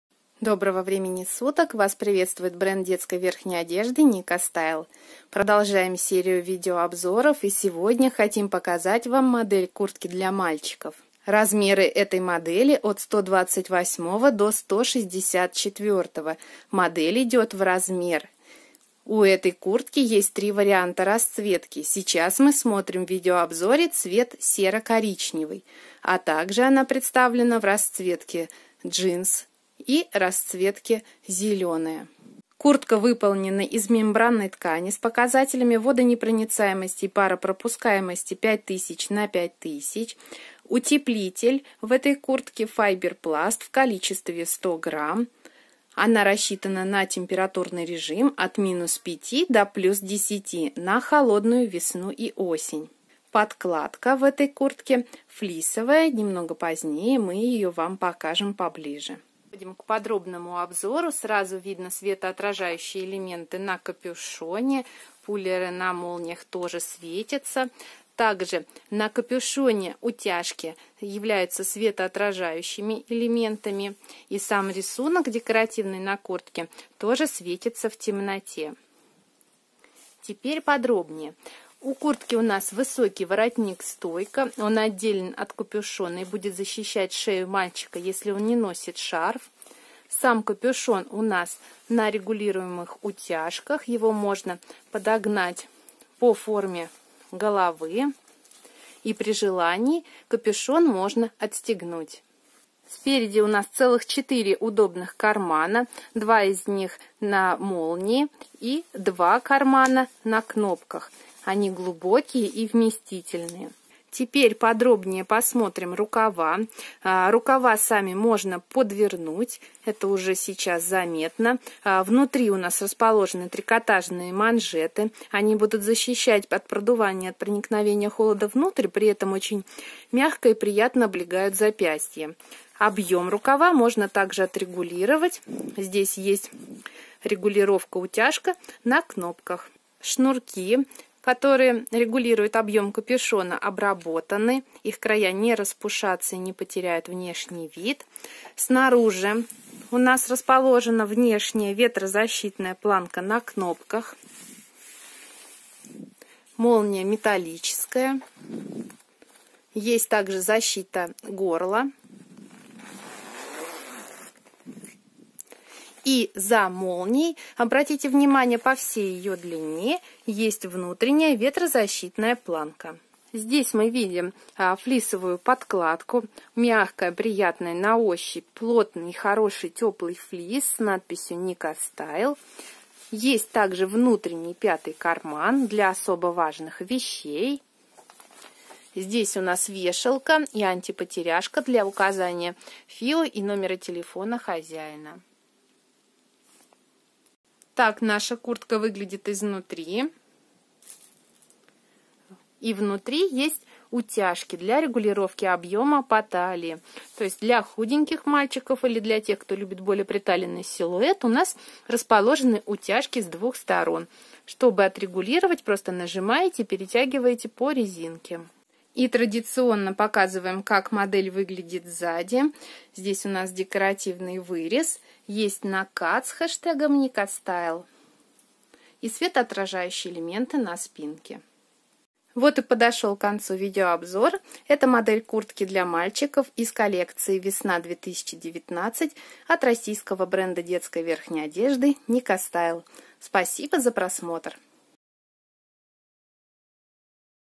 Видео-обзор куртка для мальчика 4м1219 /Коллекция Весна 2019
Температурный режим: от - 5 С до + 10 С
Ткань верха: 100% полиэстер (МЕМБРАНА Teflon Breathable WR milky 5000/5000)
Подкладка: флис (100% полиэстер)
Утеплитель: файберпласт 100 гр.